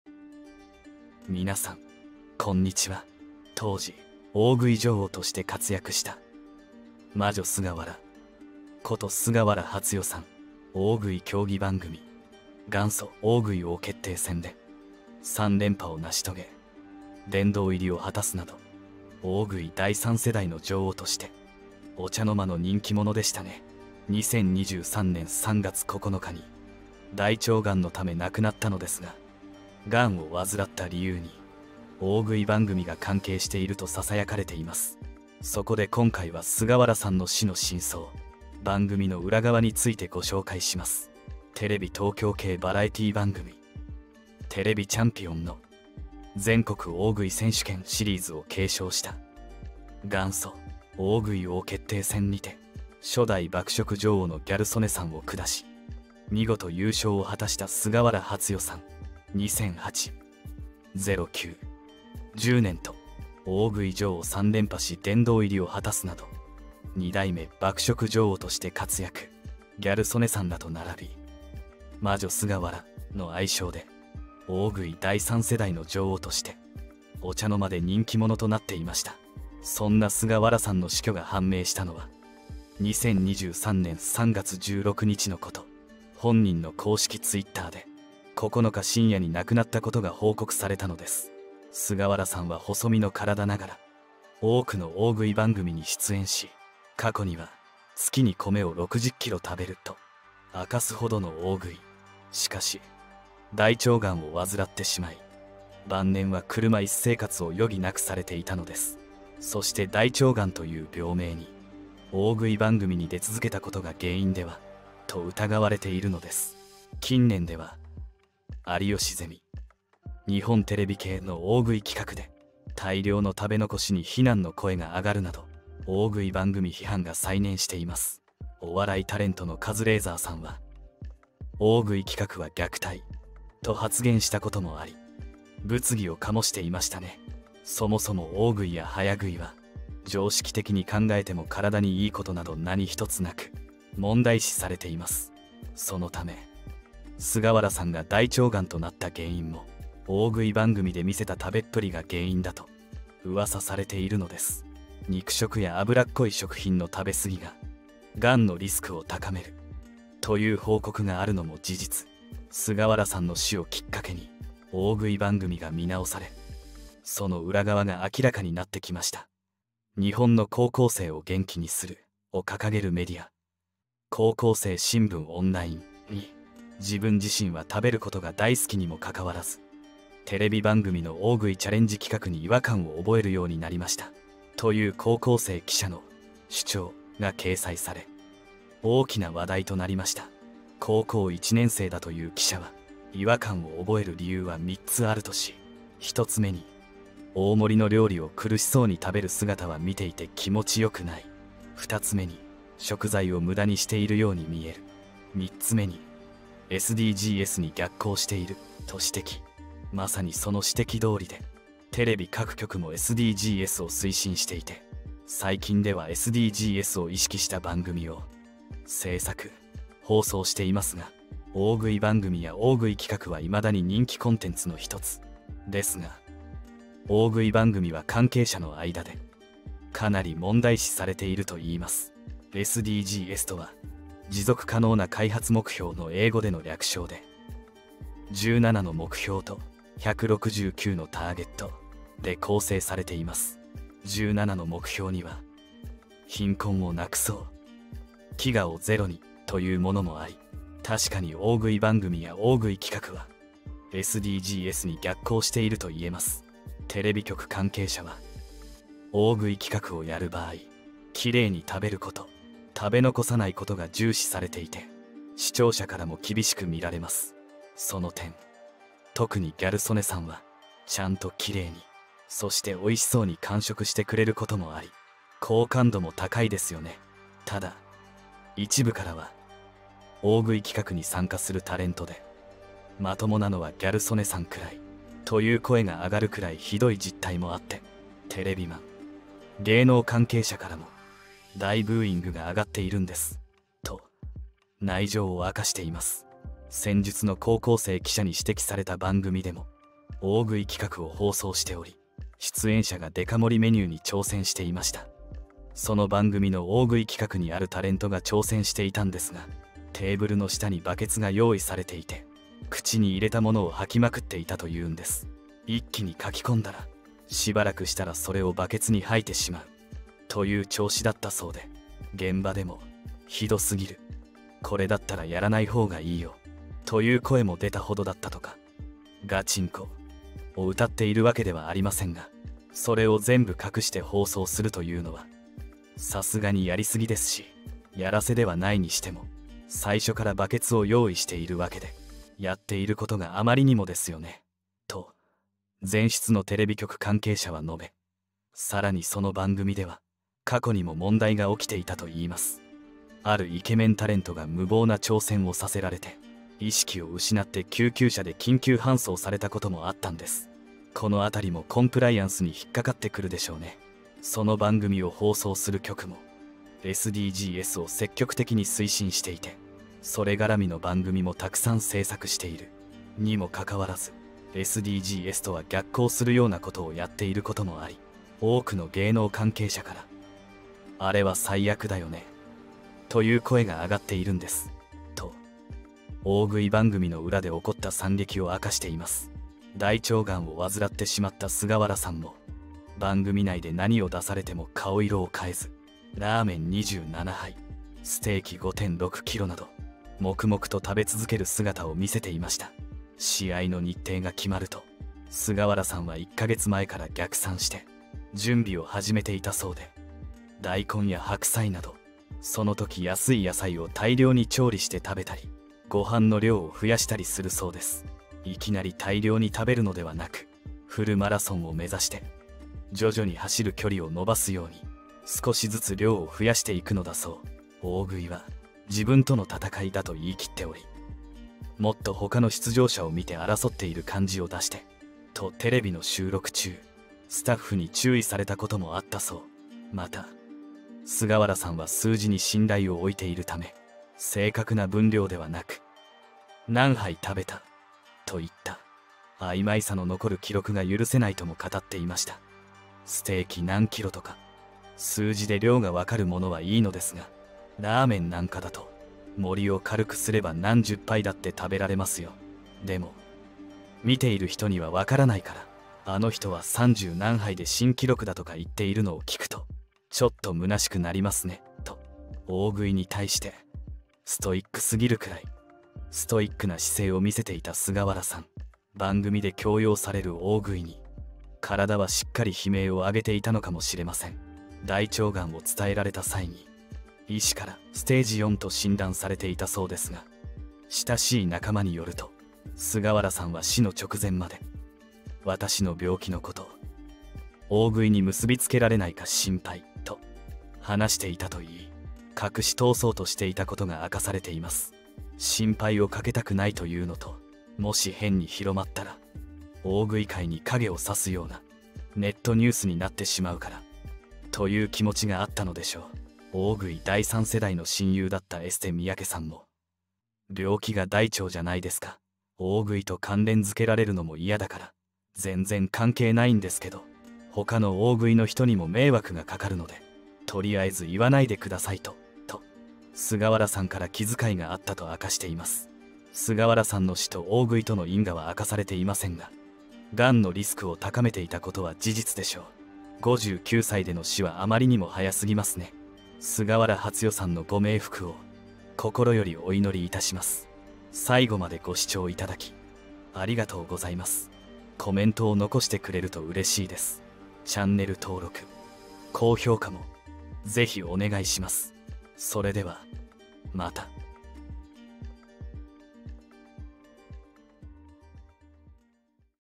菅原初代の本当の死因が発覚…身内が暴露した大食いの闇に絶句！息子の現在や”虐待”の真相に驚きを隠せない…！！【芸能】
『日刊本音』へようこそ。

当チャンネルでは、芸能人の確執に関する情報を

徹底調査してお届けしていきます。
当時「大食い女王」として活躍した

“魔女菅原”こと菅原初代さん。

大食い競技番組「元祖!大食い王決定戦」で

3連覇を成し遂げ、殿堂入りを果たすなど

大食い第三世代の女王として

お茶の間の人気者でしたね。

2023年3月9日に

大腸ガンのため亡くなったのですが

ガンを患った理由に

大食い番組が関係していると囁かれています。

そこで今回は菅原さんの死の真相、

番組の裏側についてご紹介します。